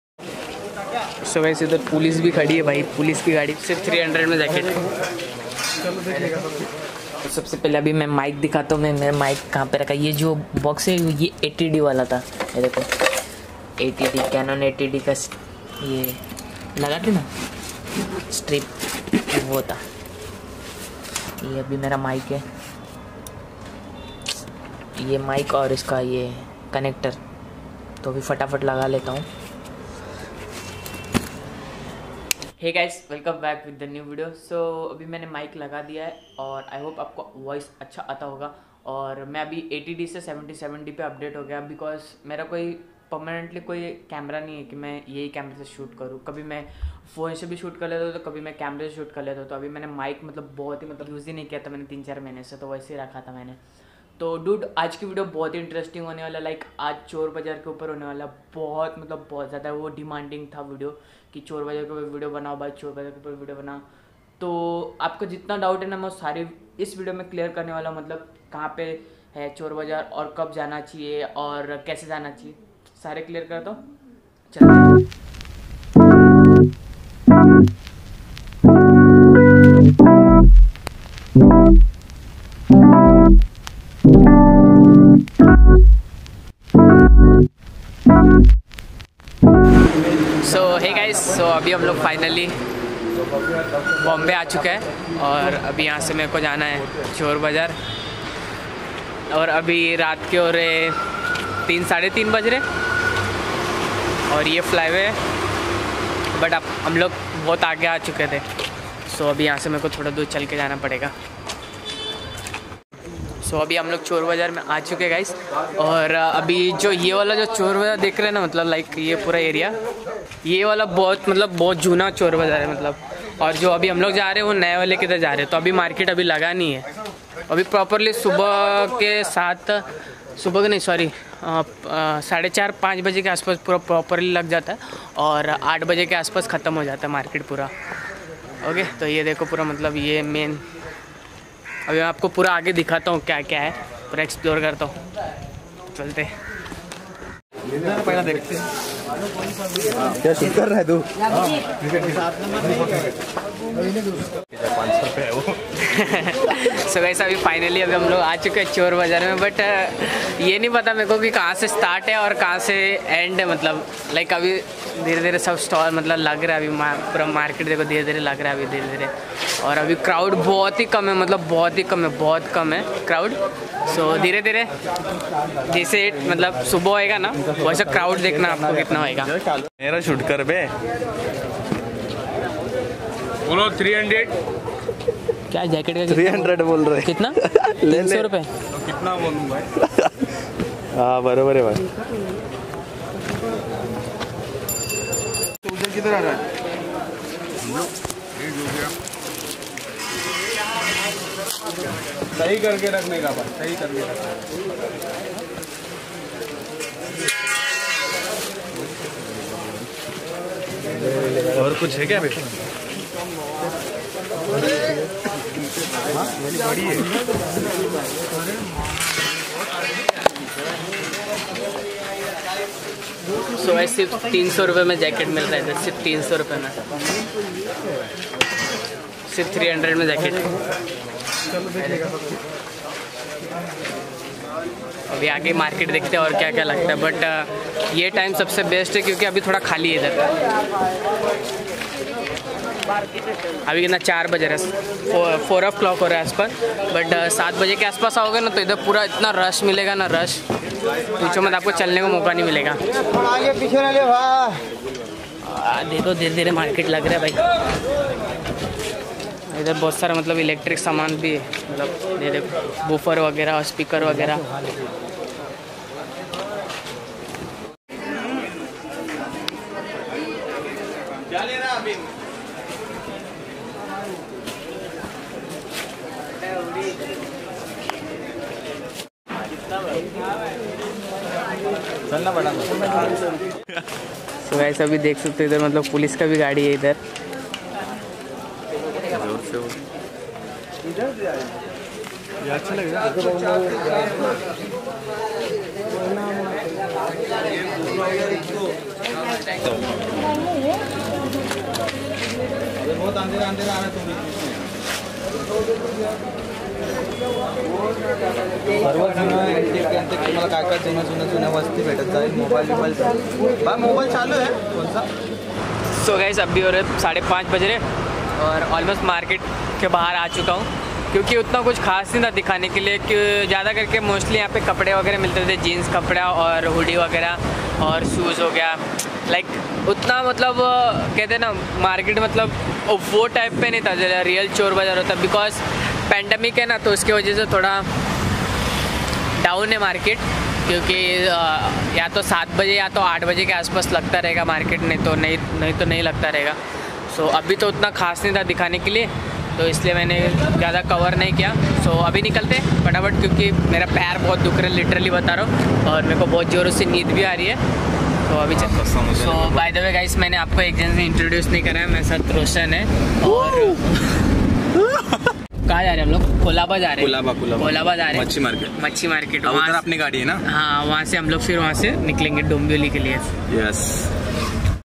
The Real Chor Bazaar 2022 || चोर बाजार में पुलिस 😡|| Night Real chor bazar vlog
chor Bazaar check location

Friday. 3 Am to 8 am time